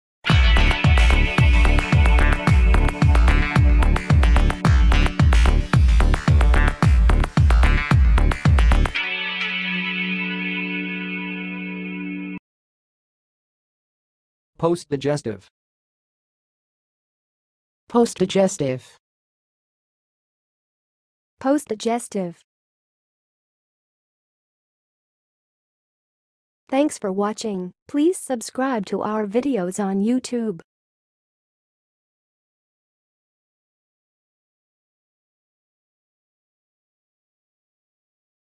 How to say "postdigestive"! (High Quality Voices)
Watch in this video how to say and pronounce "postdigestive"!